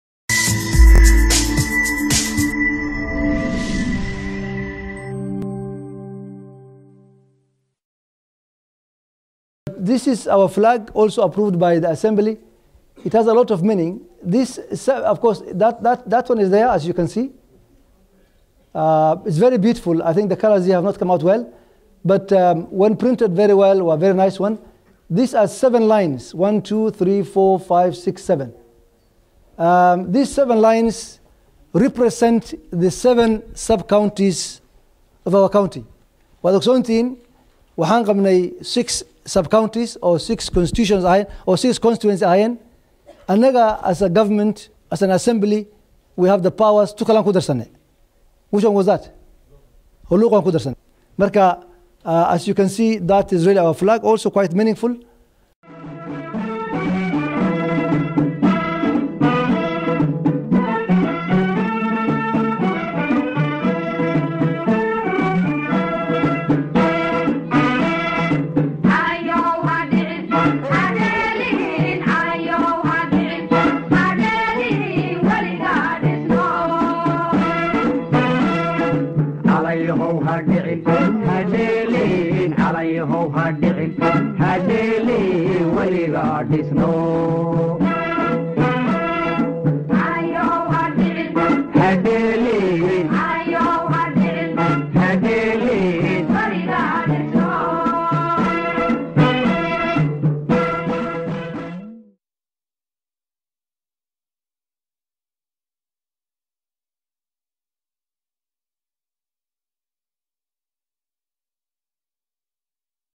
Garissa Flag - Calanka Dowlad Goboleedka Garissa - Approved By The County Assembly | 15 Dec 2014
Garissa Flag Has Seven lines, which represents
  the seven Sub-Counties of Garissa County..